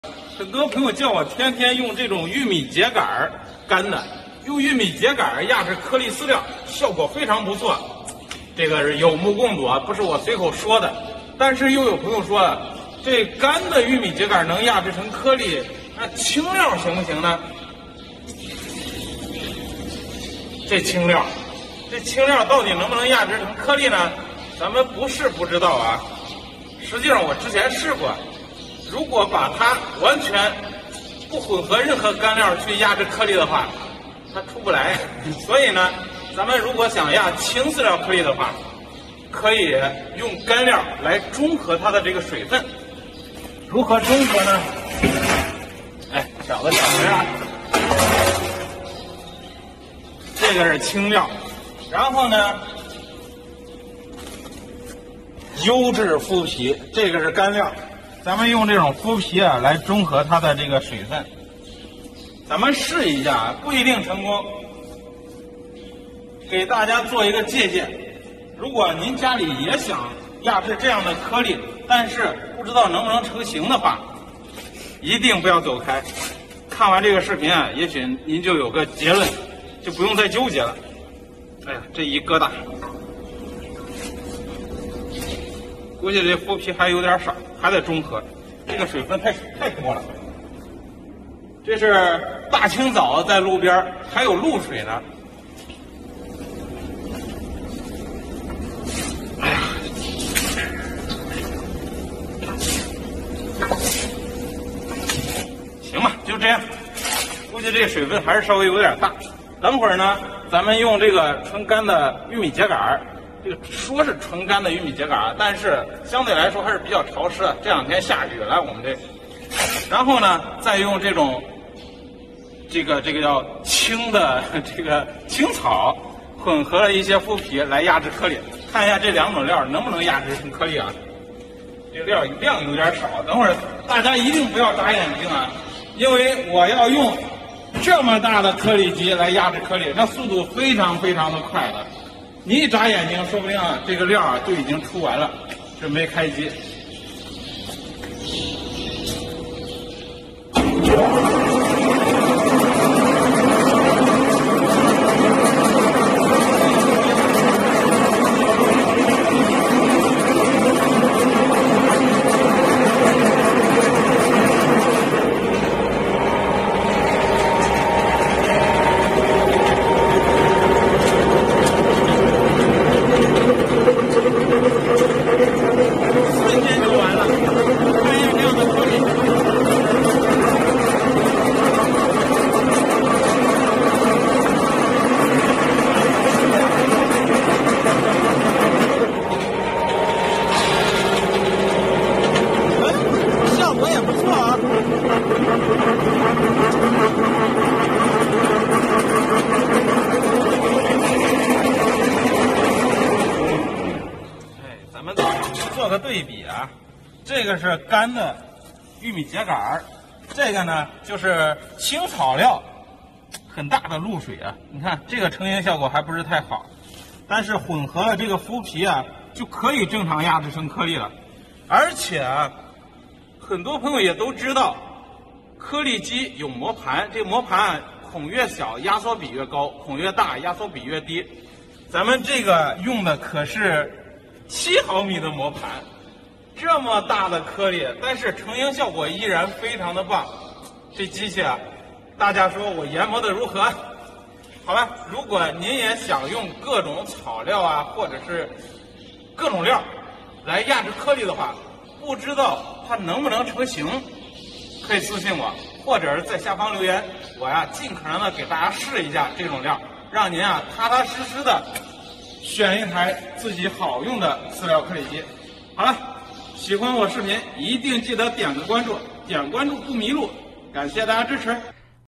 控制好含水量，青草也能做颗粒，给新疆粉丝支援一台巨无霸
控制好含水量，青草也能做颗粒，给新疆粉丝支援一台巨无霸,本视频由农人小哥儿官方原创提供,3360次播放,好看视频是由百度团队打造的集内涵和颜值于一身的专业短视频聚合平台